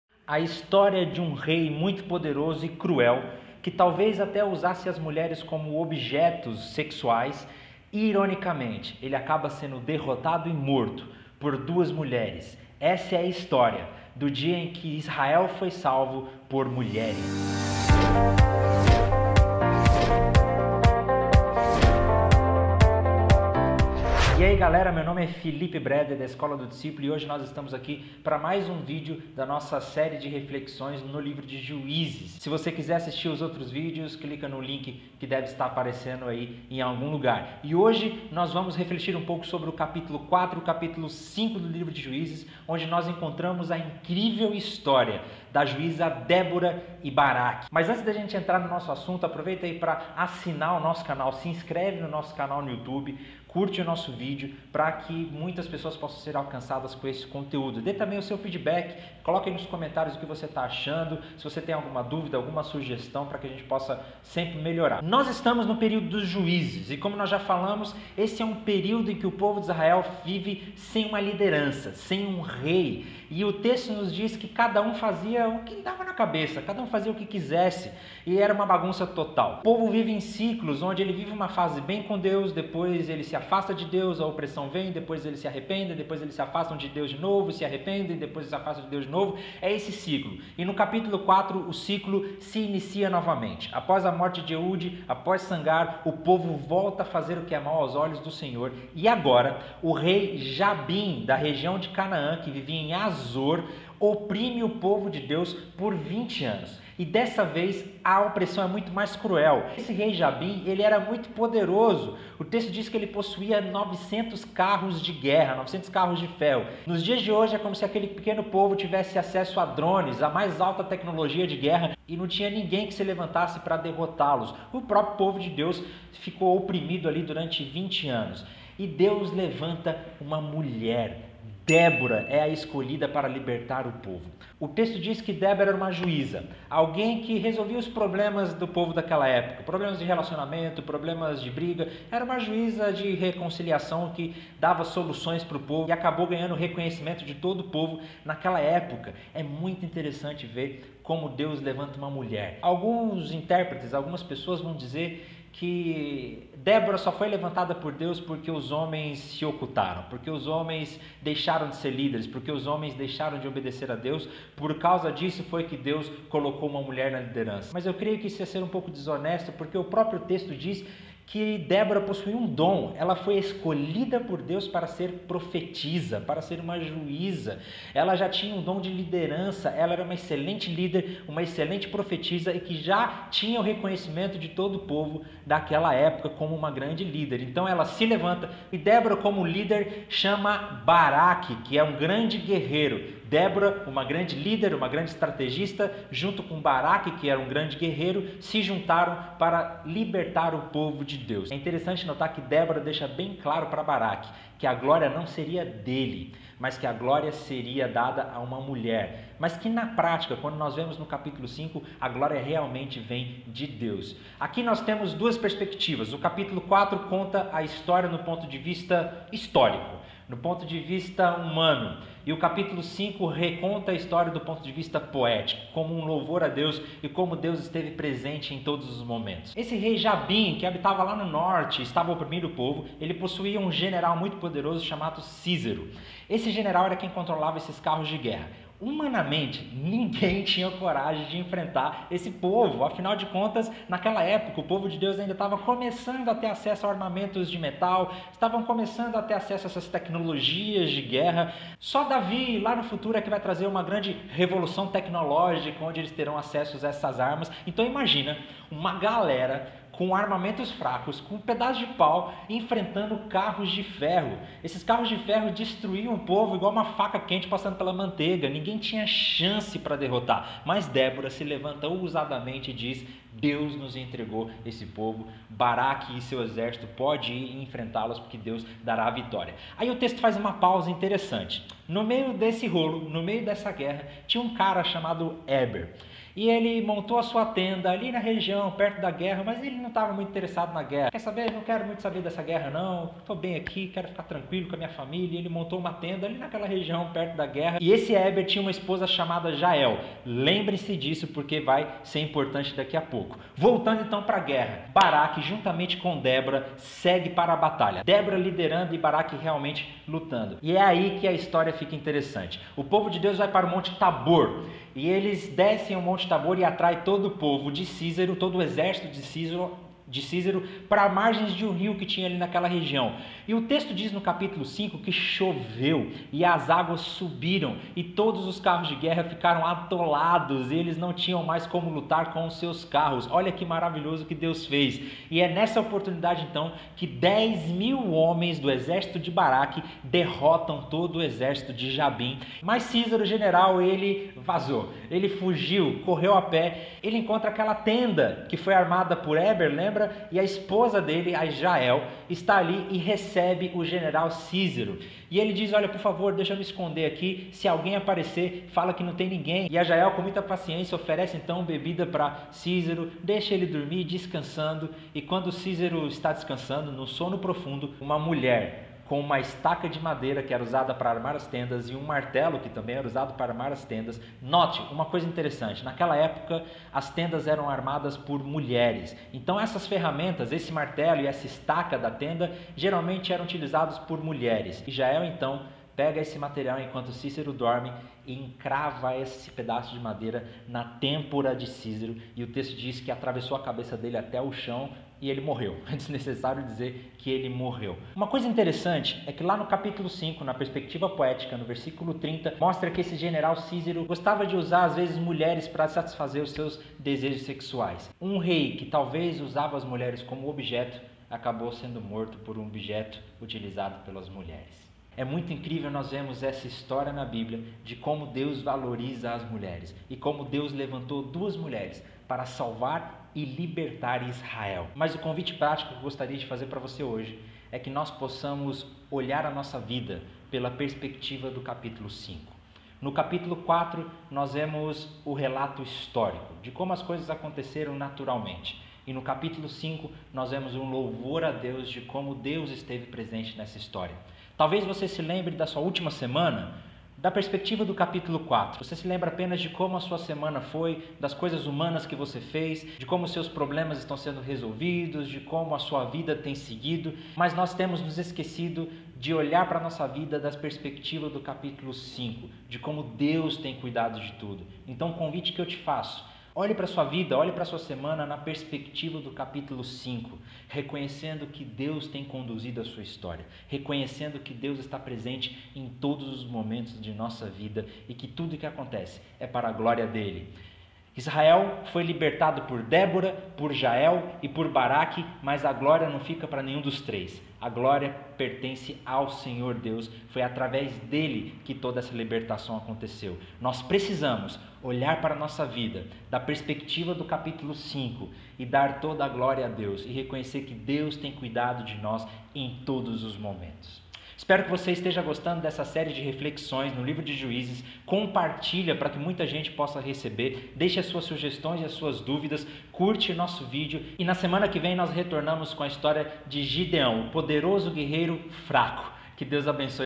Débora e Baraque - A Mulher que Salvou Israel | O Livro de Juízes Cap. 4-5 | Parte 5
Em Nosso quinto vídeo da série de reflexões no Livro de Juízes vemos a história da Juíza Débora e Baraque.

Deus valoriza as mulheres e levanta uma juíza para liderar e libertar o povo de Israel da opressão gerada pelo rei Jabim durante 20 anos.

Por: Filipe Breder

Ps. No vídeo foi citado que Baraque era um grande guerreiro, sabemos que algumas interpretações mencionam Baraque como fraco por ter pedido que Débora o acompanhasse. Existe uma outra interpretação de que Baraque pediu que Débora o acompanhasse justamente porque a respeitava e a via como líder. Independente da interpretação adotada, ao final Baraque foi sim um grande guerreiro que derrotou o exército de Sísera.